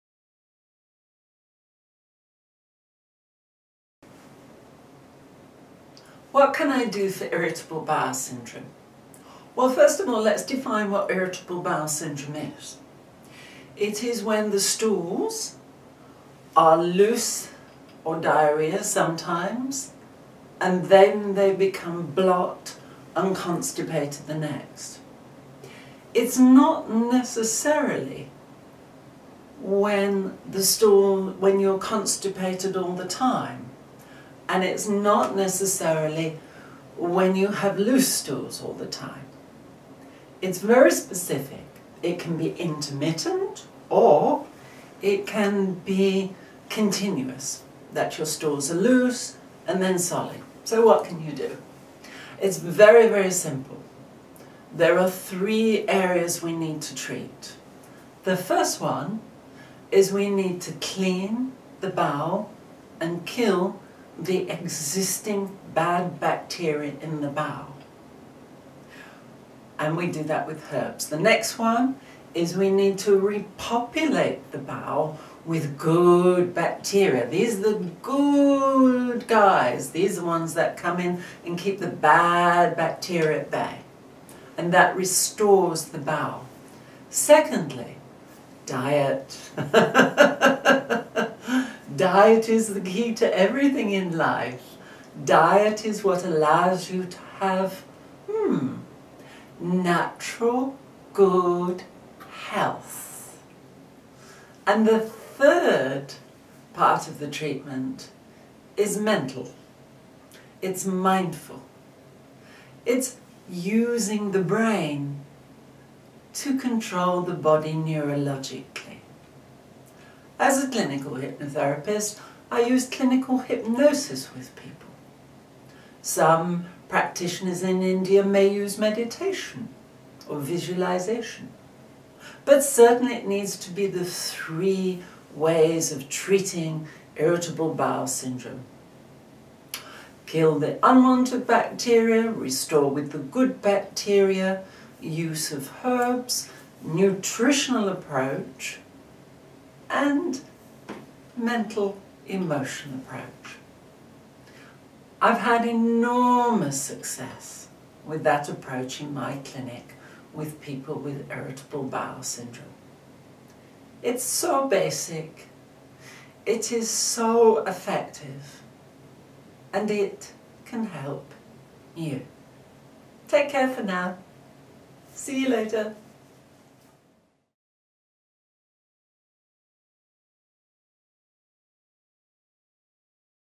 What Can I Do For Irritable Bowel Syndrome by Dr. Tracie O'Keefe DCH, BHSc, ND
What Can I Do For Irritable Bowel Syndrome?
by Dr. Tracie O'Keefe DCH, BHSc, ND

Well, first of all, let us define what irritable bowel syndrome is. It is when the stools are loose (or Diarrhoea) sometimes, and then they become blot and constipated the next. It is not necessarily when you are constipated all the time, and it is not necessarily when you have loose stools all the time. It is very specific - it can be very intermittent or continuous. That your stools are loose, and then solid. So what can you do? It is very simple. There are 3 areas we need to treat.

The first one is we need to clean the bowel and kill the existing bad bacteria in the bowel, and we do that with herbs; then, we need to re-populate the bowel with good bacteria. Second, adjust your diet. Diet is the key to everything in life. Diet is what allows you to have natural good health; and the third part of the treatment is mental -- it's mindful. It's using the brain to control the body neurologically.

As a clinical hypnotherapist, I use clinical hypnosis with people. Some practitioners in India may use meditation or visualisation. Certainly, it needs the 3 ways of treating irritable bowel syndrome.